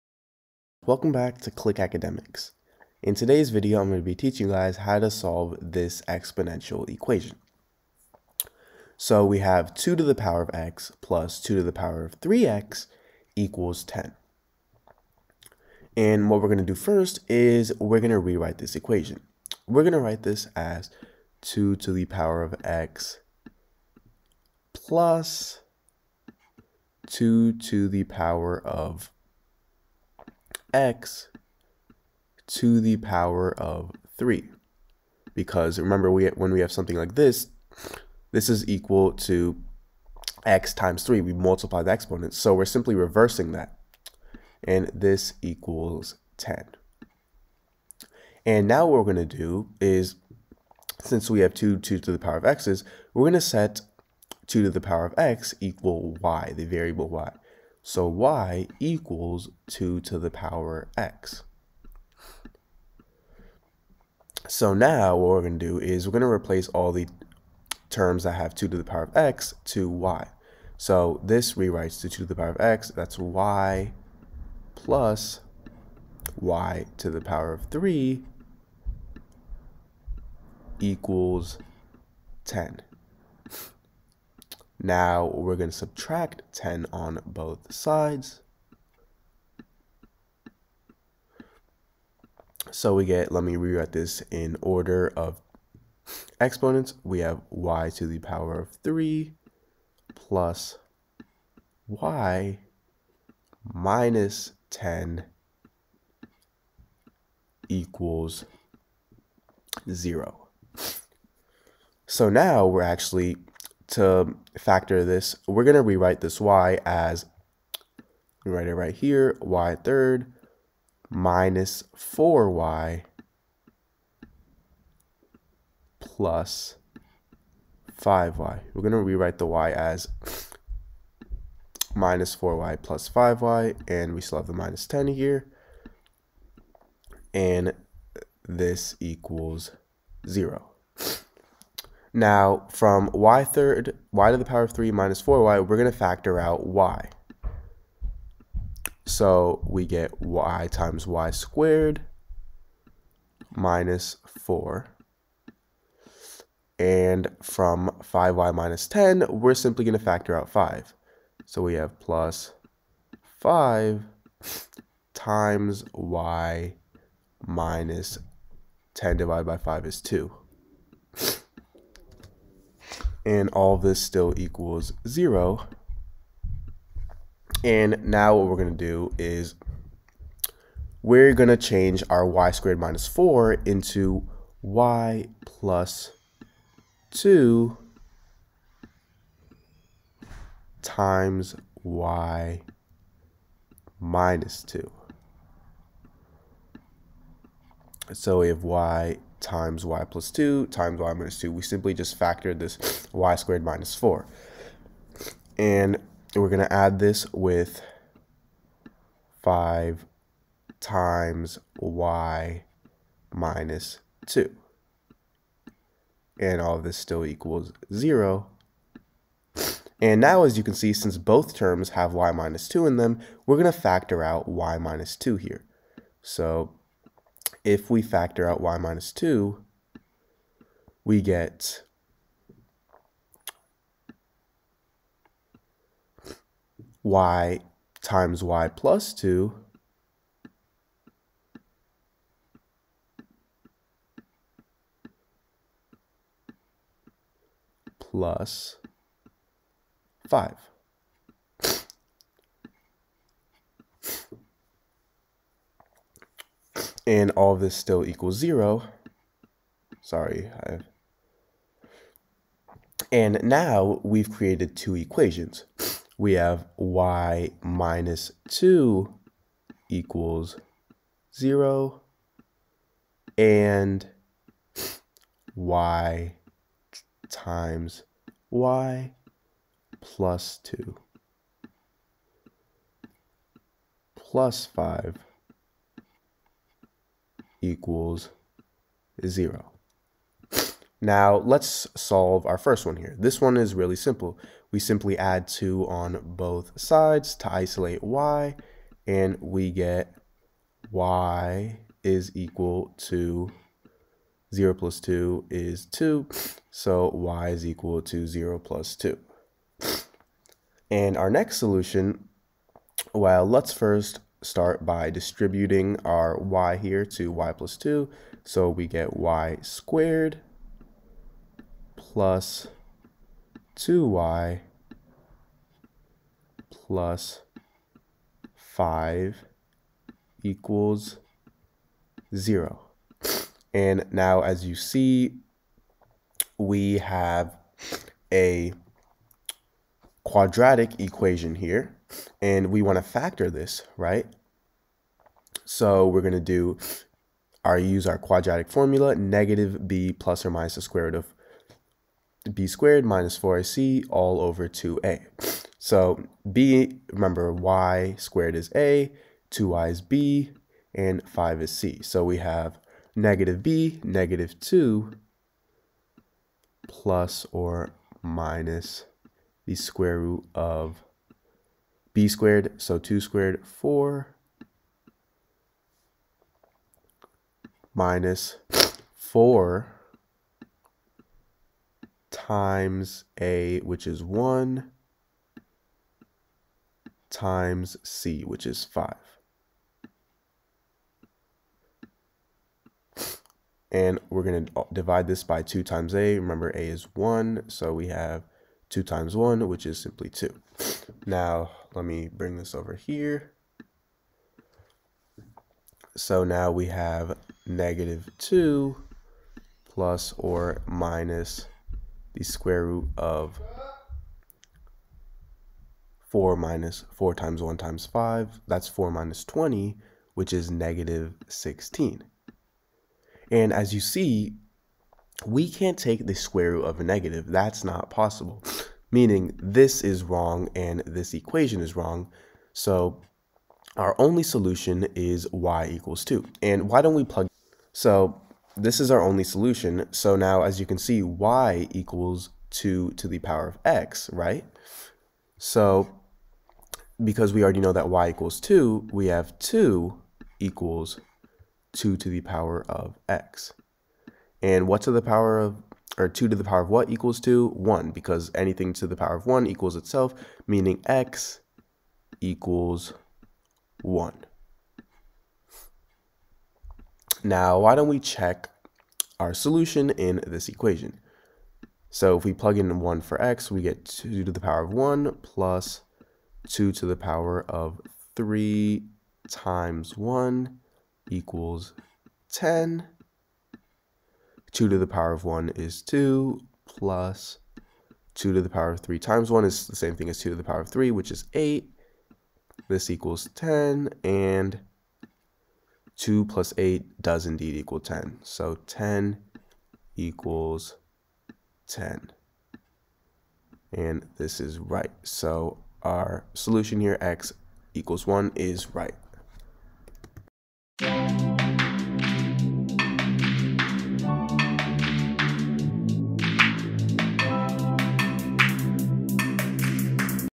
A Nice Exponential Equation
In todays video, we will be teaching you how to solve an exponential equation.